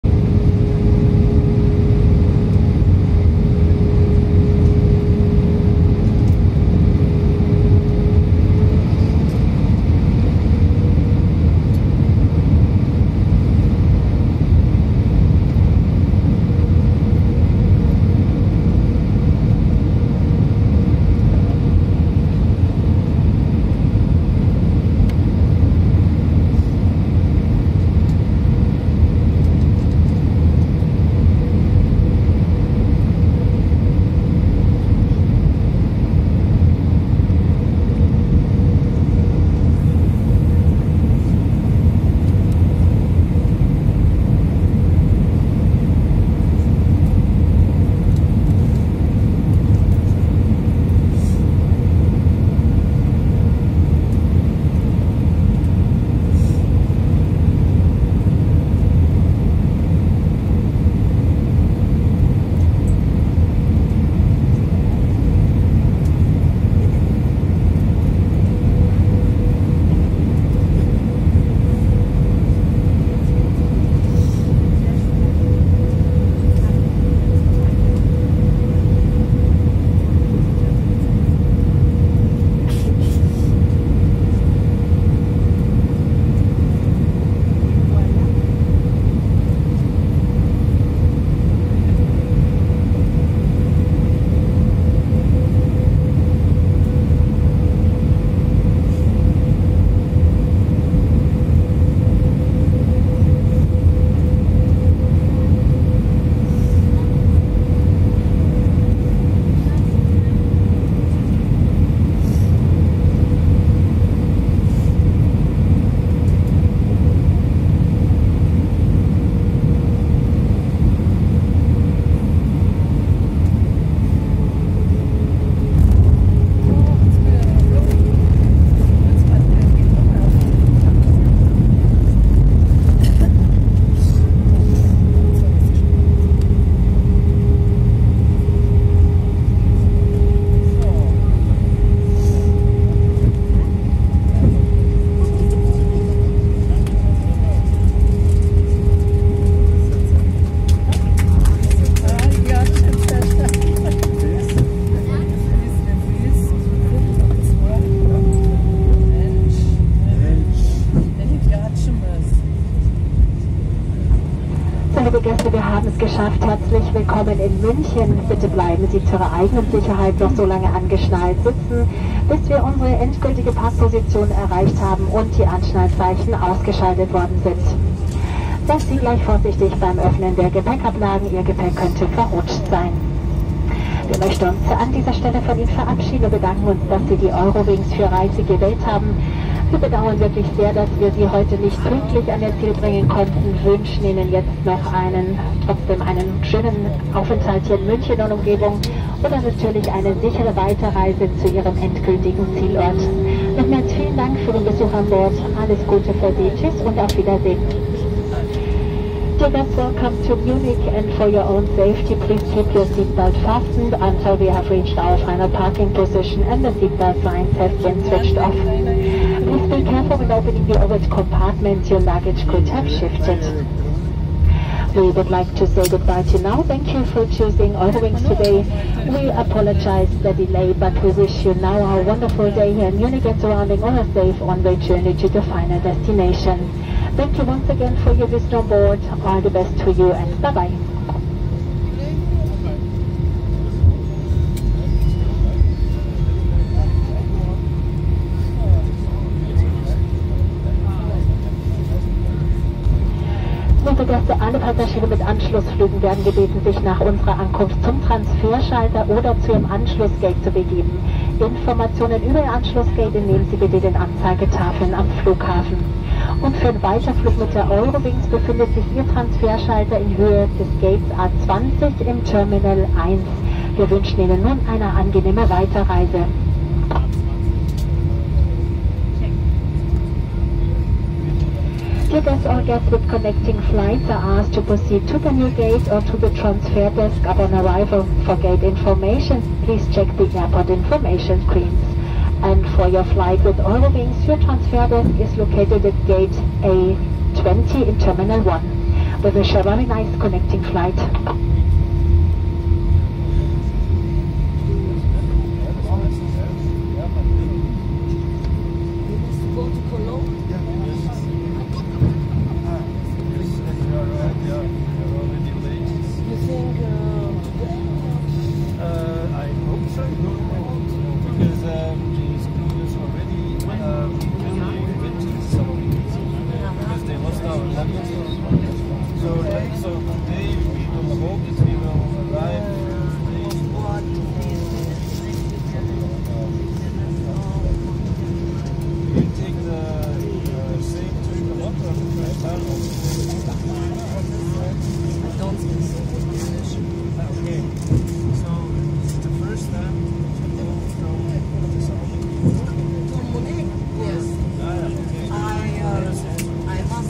Eurowings A320 Landing in Munich - Wing View
Eurowings A320 landing and taxiing to parking in Munich. Wing view.